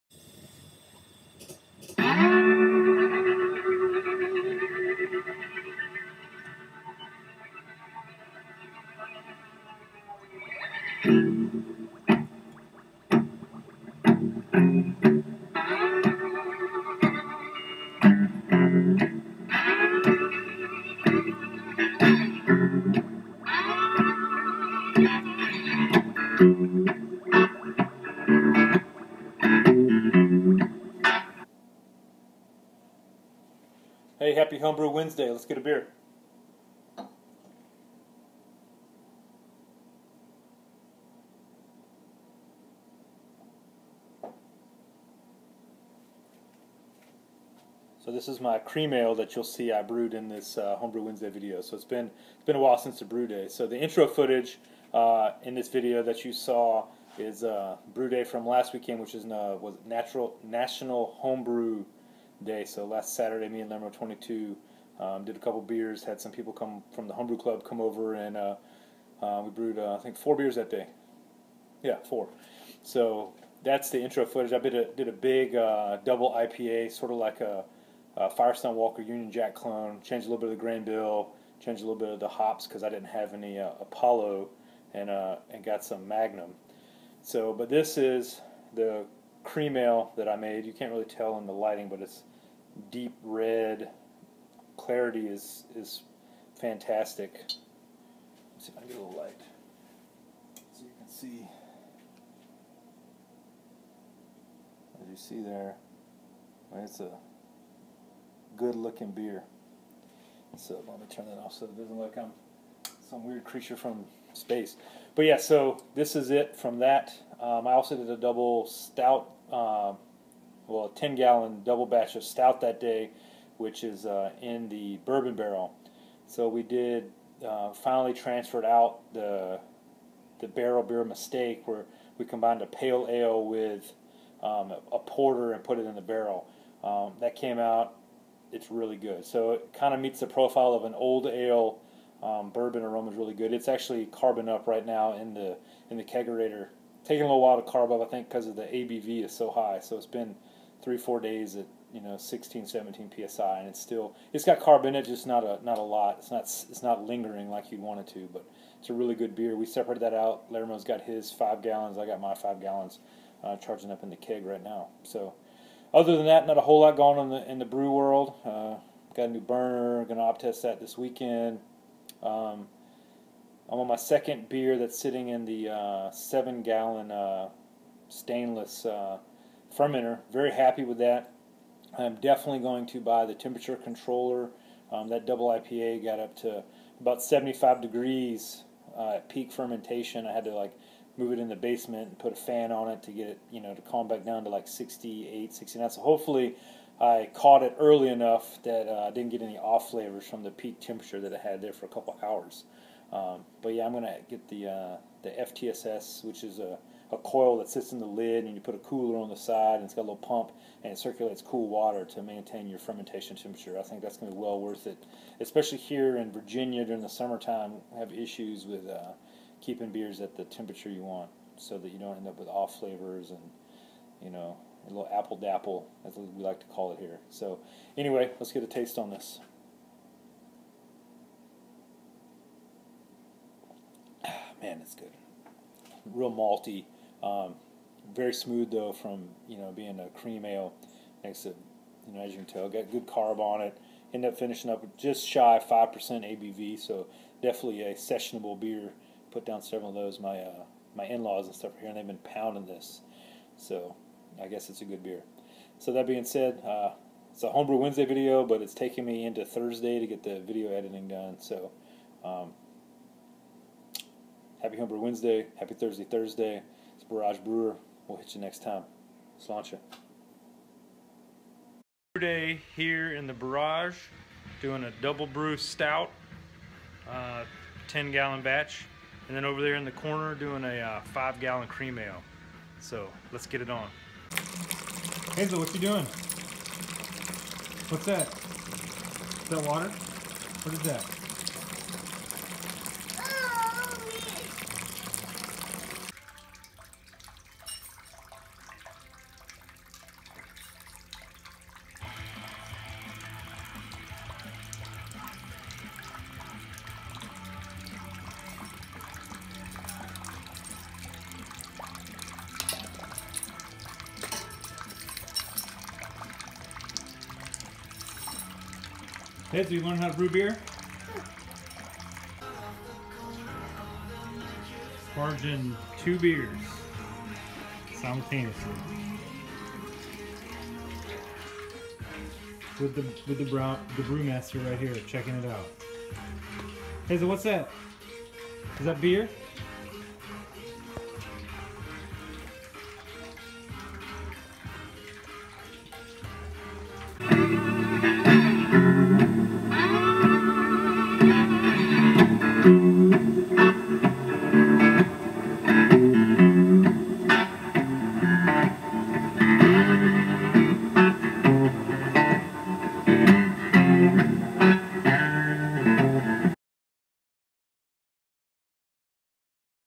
HBW51017 - Irish Cream Ale
HBW51017 - discussing my cream Ale and brew day footage